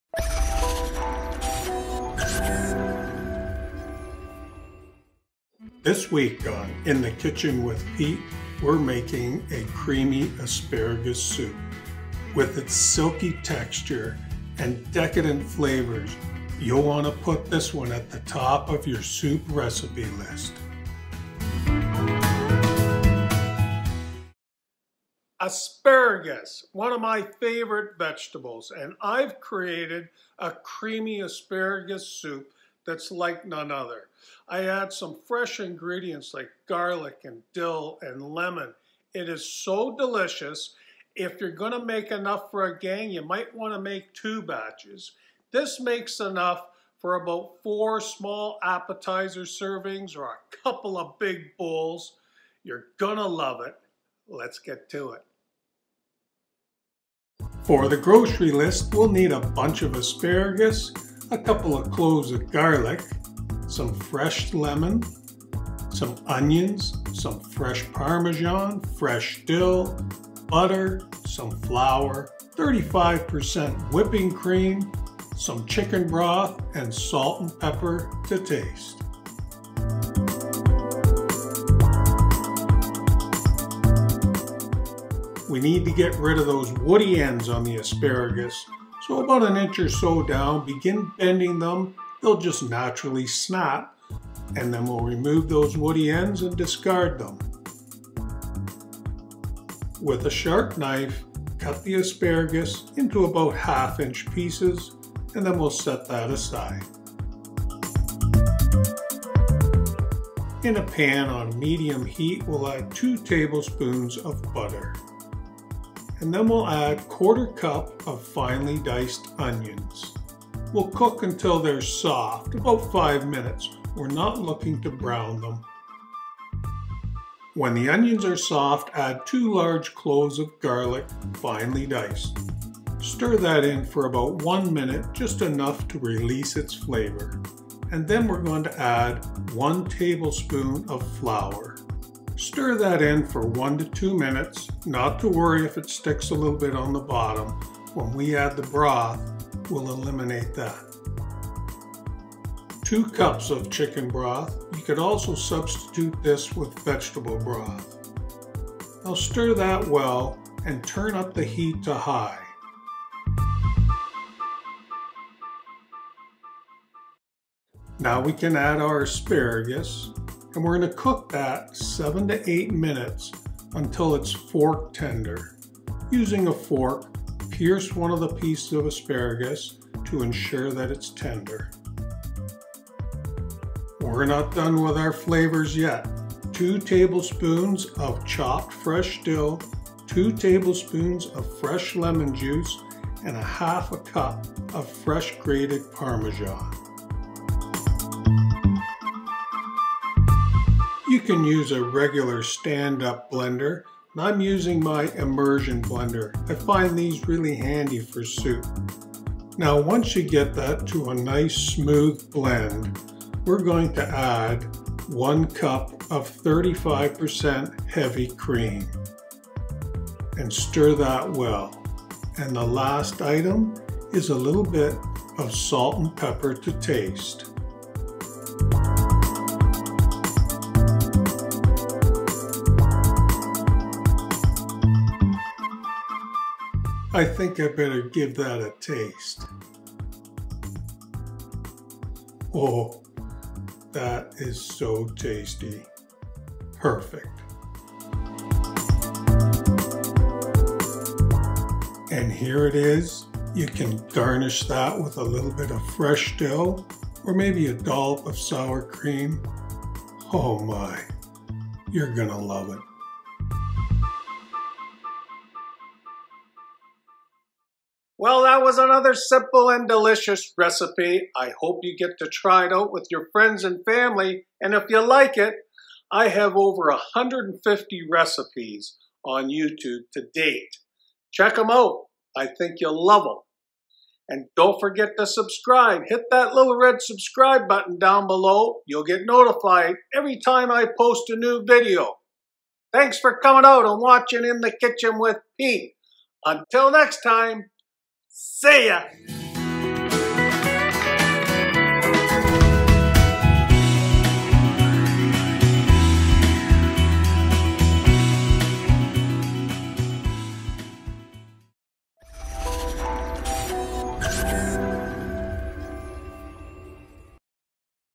Creamy Asparagus Soup Ep 155
Creamy Asparagus Soup Recipe:
Prep:
- 1 bunch of asparagus diced into 1/2" pieces, woody ends removed and discarded.
- 1/4 cup of finely diced white onions
- 2 large cloves of garlic finely diced
- 2 Tbs. fresh squeezed lemon juice
- 2 Tbs. fresh chopped dill
- 1/2 cup of fresh grated parmesan cheese
- measure 2 cups of chicken broth ( can sub with vegetable broth )
- measure 1 cup of 35% heavy cream
Cooking:
- in a pan on medium heat add 2 Tbs. of butter
- add 1/4 cup of onions, cook until soft ( 5 minutes ) not brown
- stir in the 2 cloves of garlic, cook 1 minute
- stir in the 2 Tbs. of flour, cook 2 minutes
- stir in 2 cups of chicken broth, turn up to high temperature
- stir in diced asparagus, cook 7-8 minutes until fork tender
- turn heat down to low
- stir in chopped dill
- stir in fresh lemon juice
- stir in fresh grated parmesan cheese
- puree with immersion blender until smooth
- stir in heavy cream
Garnish with fresh dill and/or a dollop of sour cream or plain yogurt
Pete.